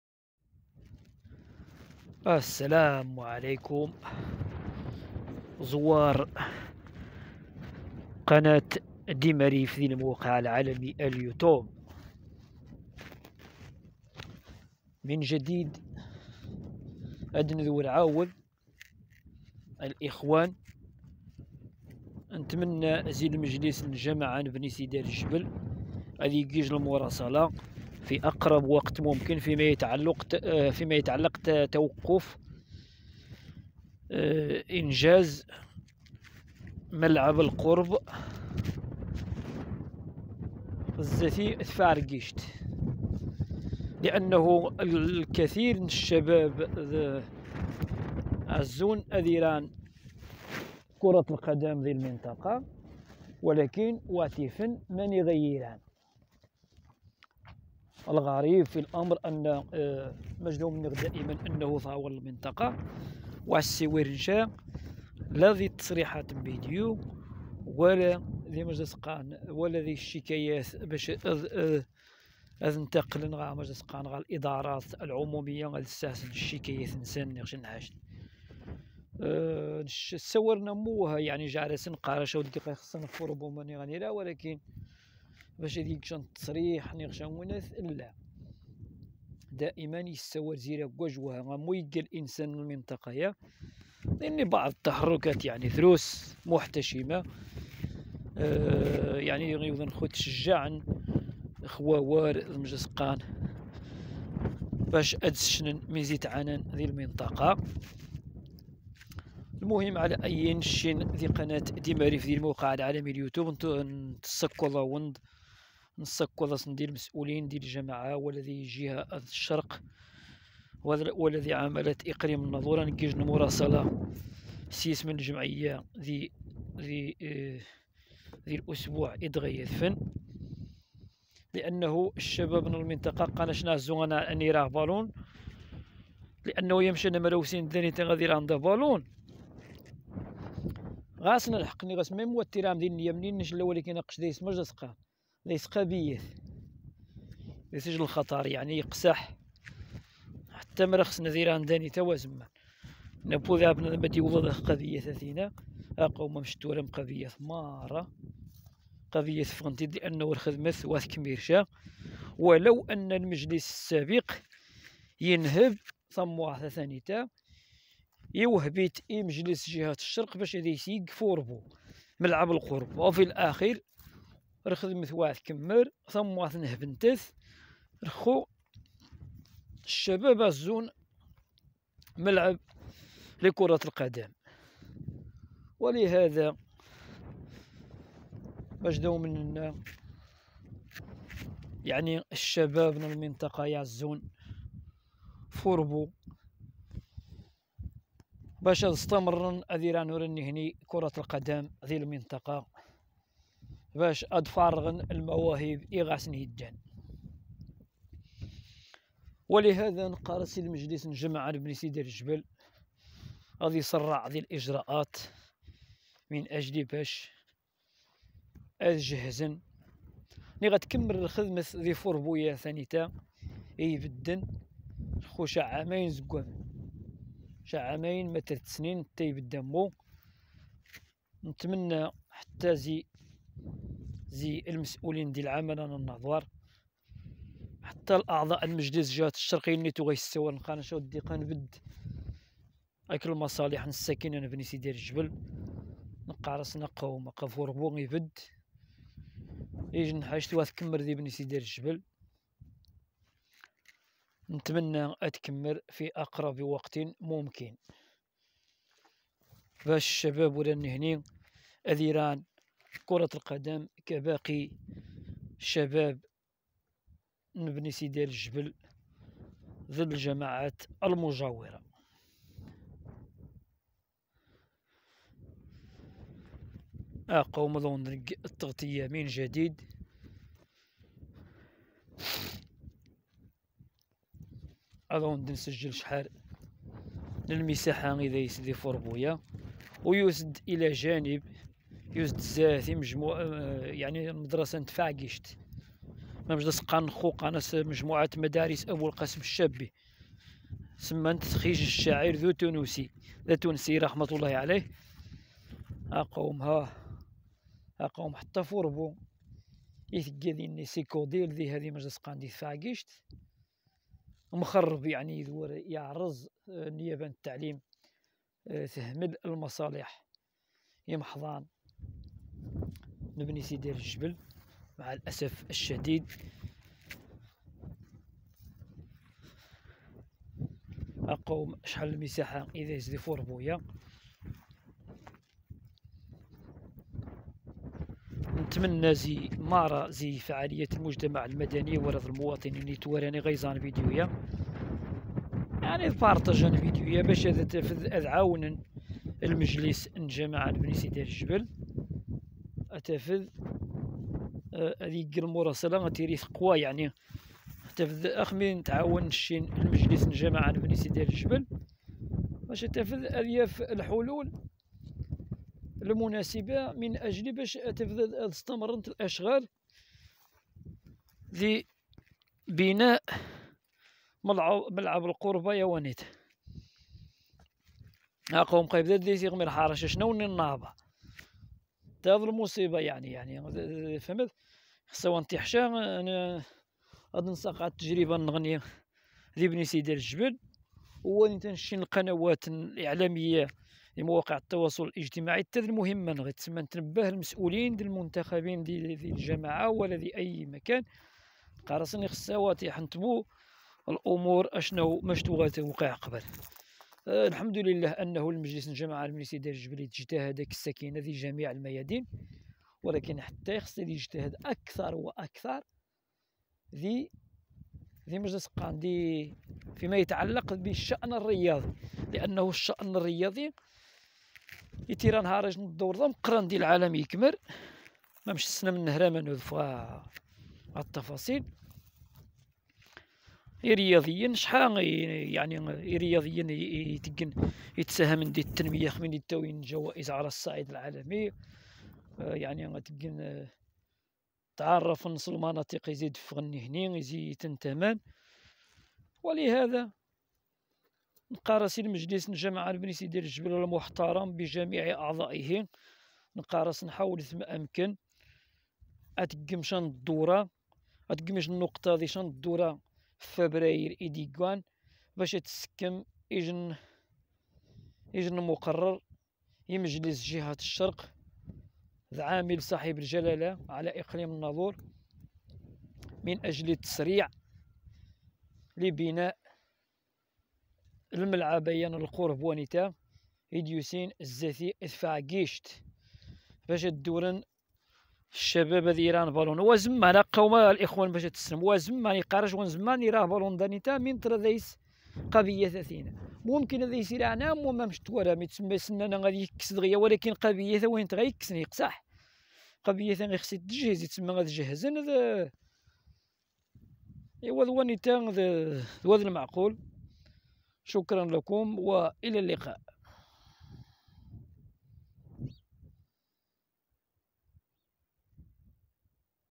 شباب بني سيدال الجبل ضائعون ــ و هذا ما حدث قرب ثفركيشت ـ لا إله إلا الله وحده لا شريك له ،
غموض يلف مشاريع مجلس جهة الشرق ببني سيدال الجبل ضواحي الناظور